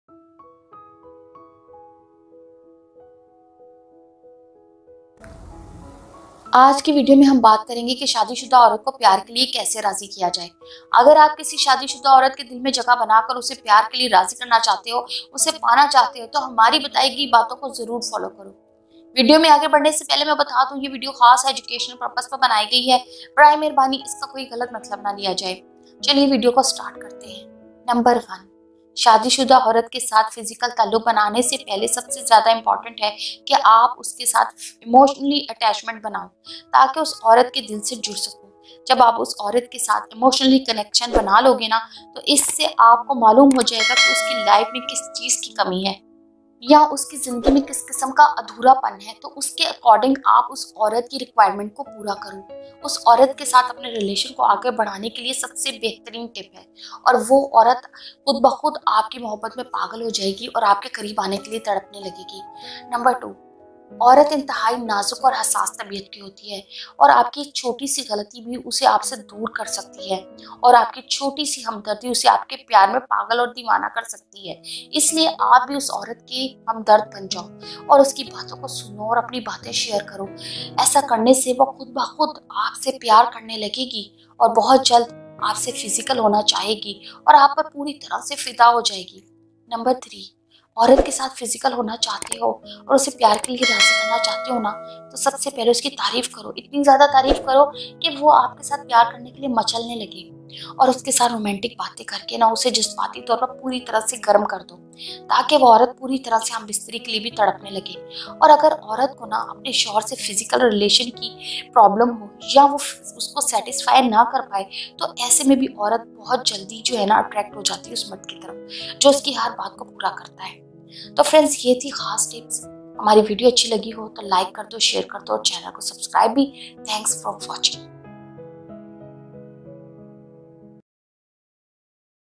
Shadi Shuda Aurat Ko Razi Karne Ka Tarika || Zeeha Daily
In this video, I have told you three such tricks that you can make any married woman fall in love with you. If you want to konw the married woman agree to love with you. must watch the video.